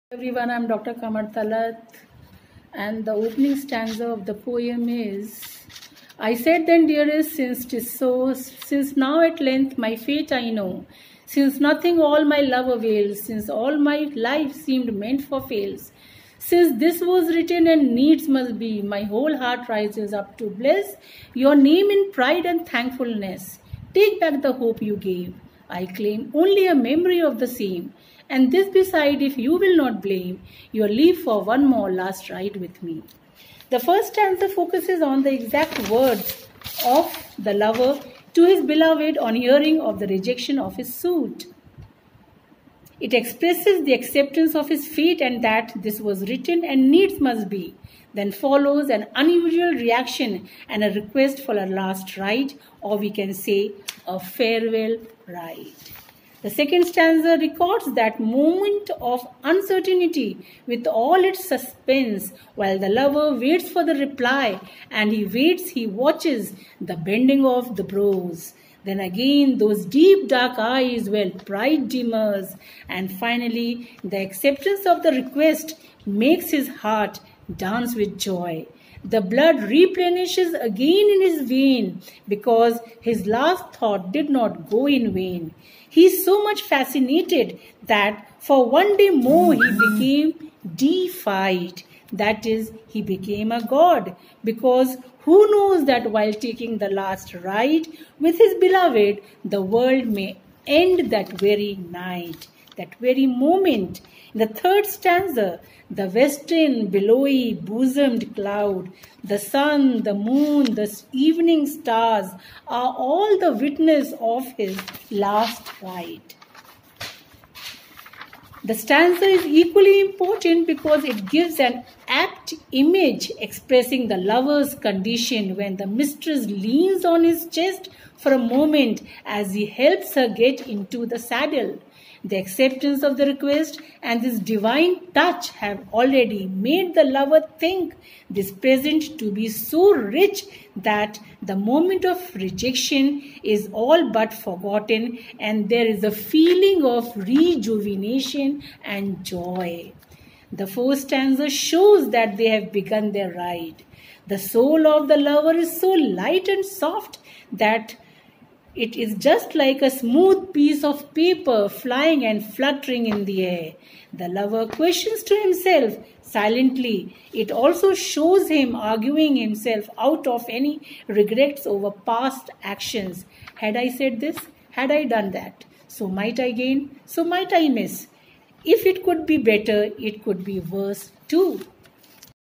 Analyzing Robert Browning poem The Last Ride Together Part two
This video is made for Post Graduate students of English Literature.